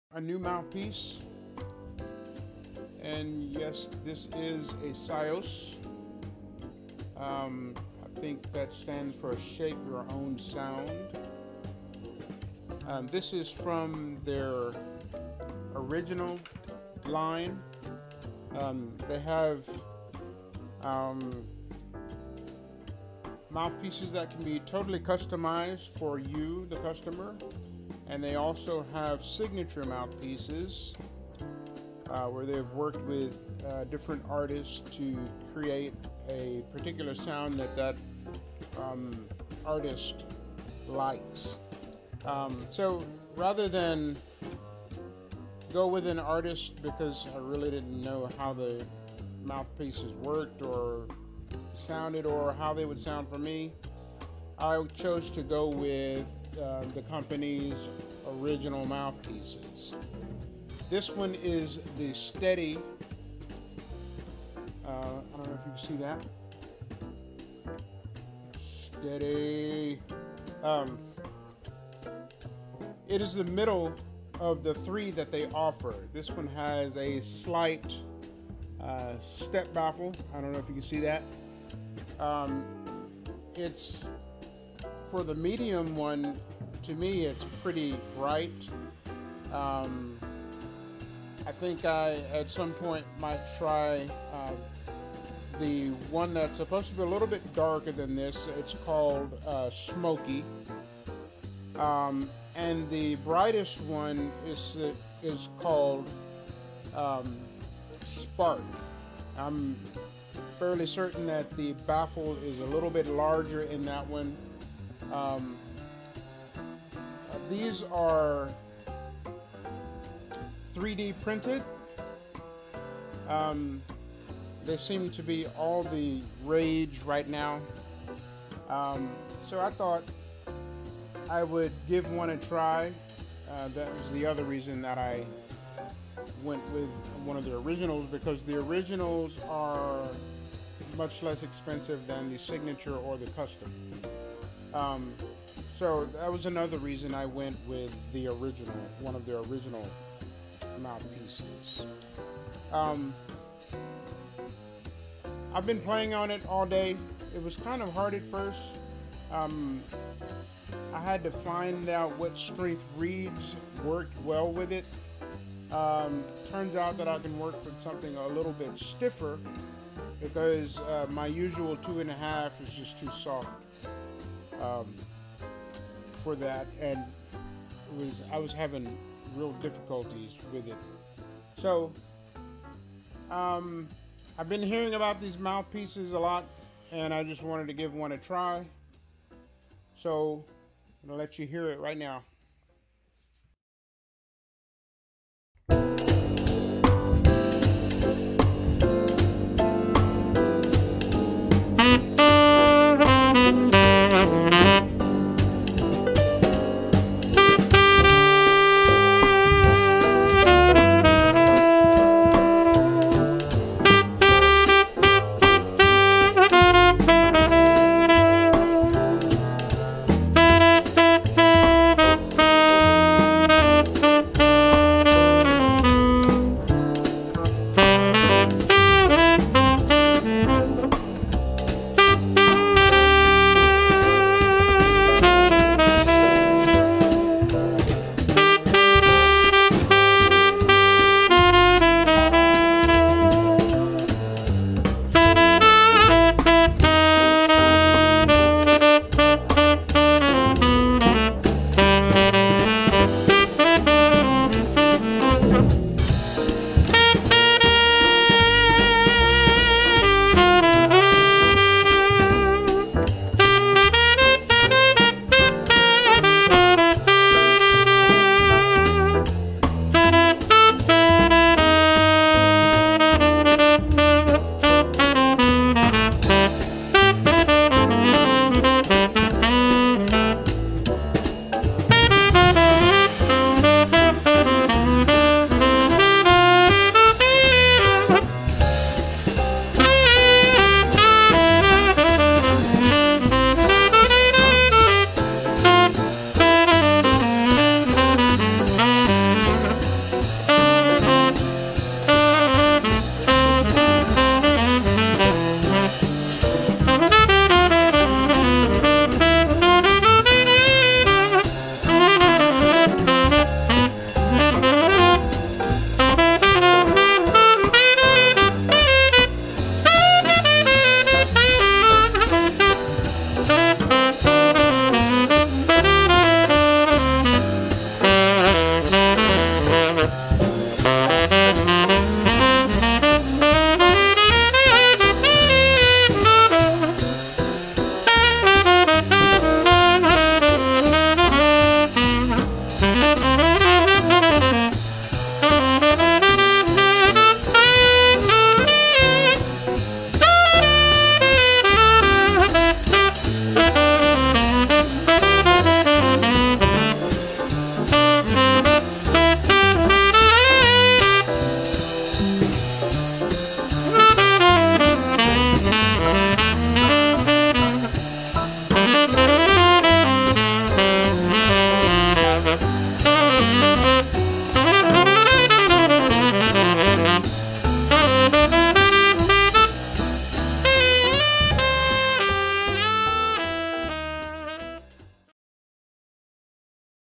SYOS Steady Mouthpiece
I was looking forward to trying out the SYOS (shape your own sound) mouthpiece, and I finally got hold of one. It sounds great, but I don't think it is as effortless as everyone says. For me, it required a stiffer reed. Anything softer, and it does not sound quite as nice. It makes me work very hard. That being said, I like the piece and will probably continue to use it.